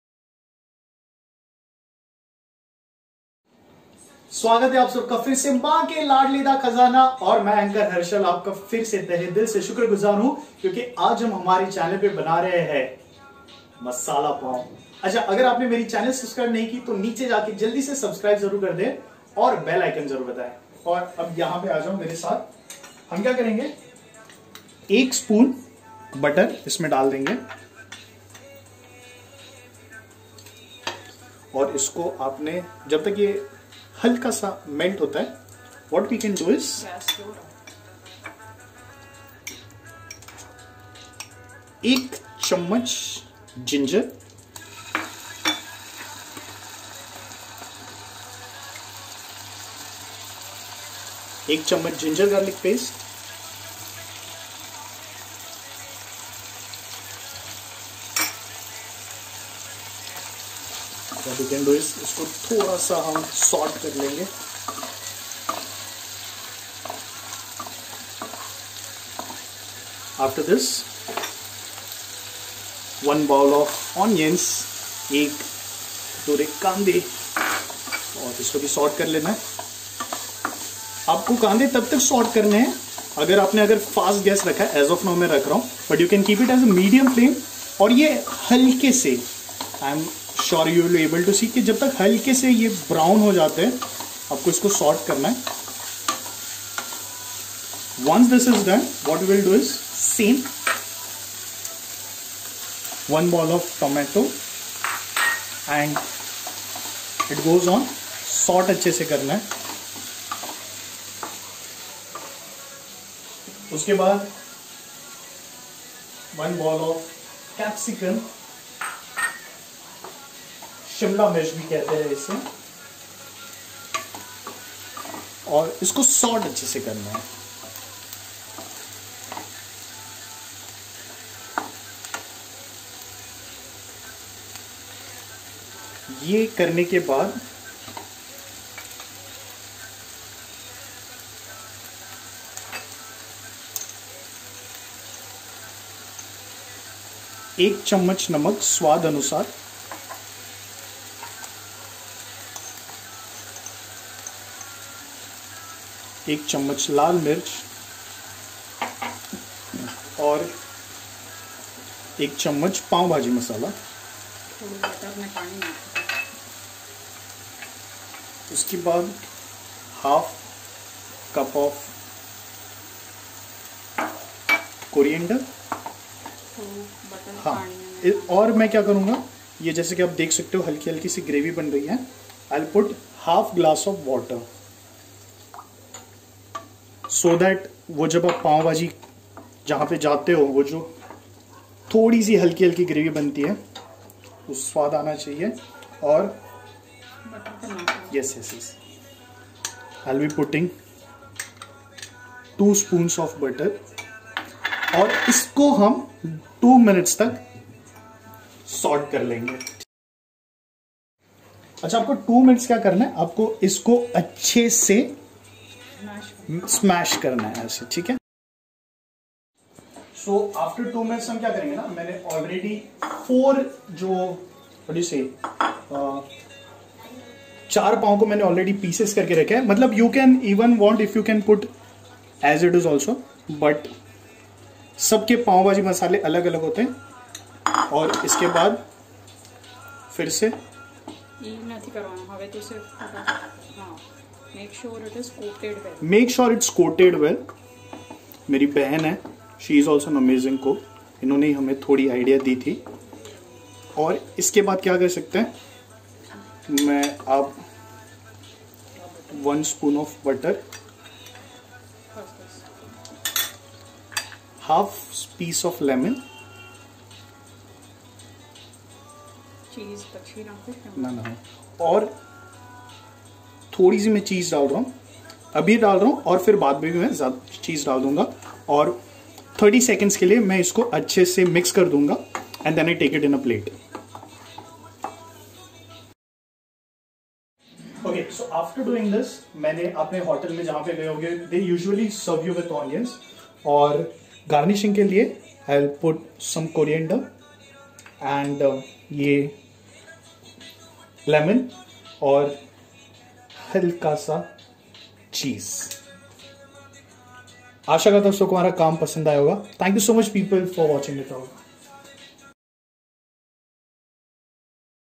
Masala Pav | Cheese Masala Pav | How to make Masala Pav at Home ? | Masala Pav Recipe|Anchor Harshal
Ingredients :
1 spoon Butter,1 spoon Ginger,
1 spoon Ginger Garlic Paste,
1 bowl of onion,tomatto,Capsicum.

1 spoon salt,1 spoon Laal Mirch,1 spoon Pav Bhaji Masala,
1/2 cup corriander,1/2 glass of water,
2 spoon of butter